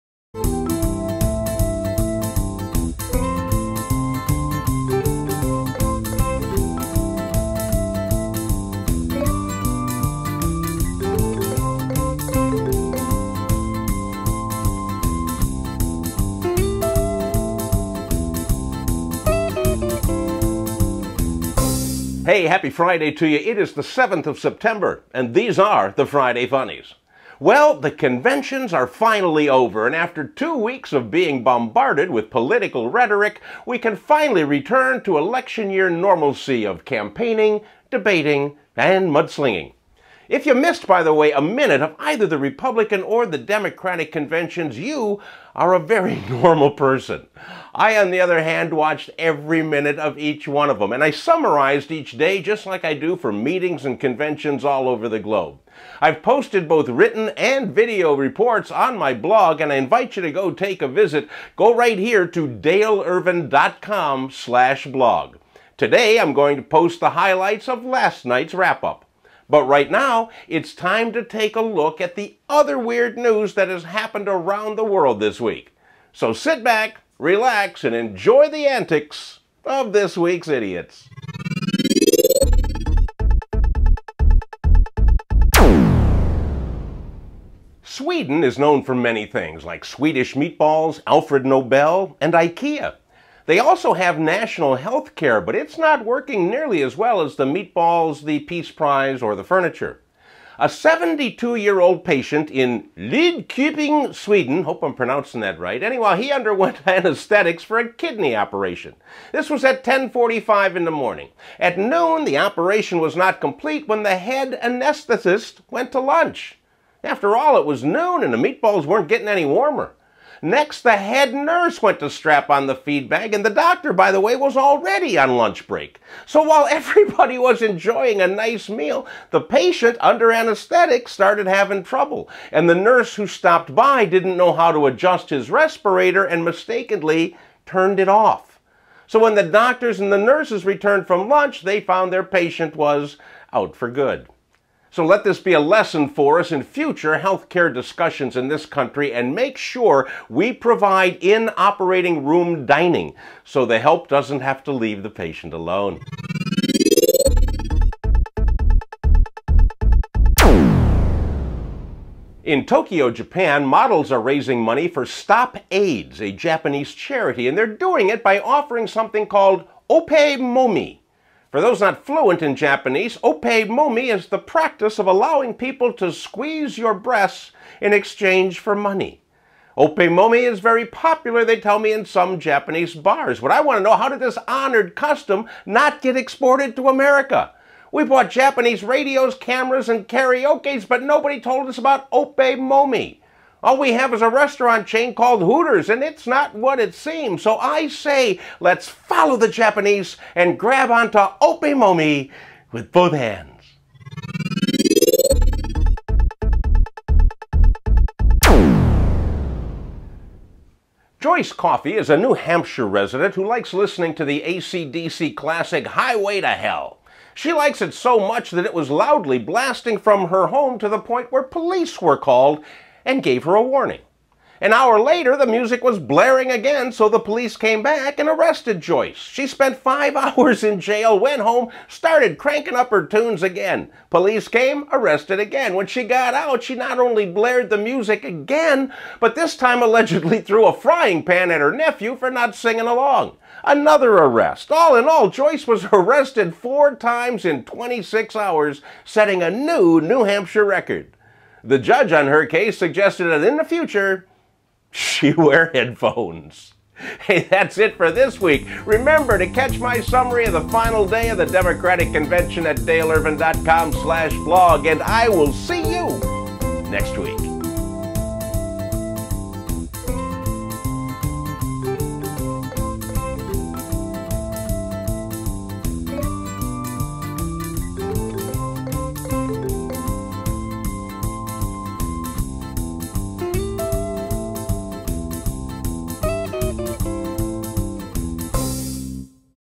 Dale Irvin's Friday Funnies - September 7, 2012
Now that the conventions are over,we can take a look at the more ridiculous stories of the week. This week we look at healthcare gone terribly awry; the best charity fundraiser I have ever seen; and what happens when old people lose their hearing but not their musical taste.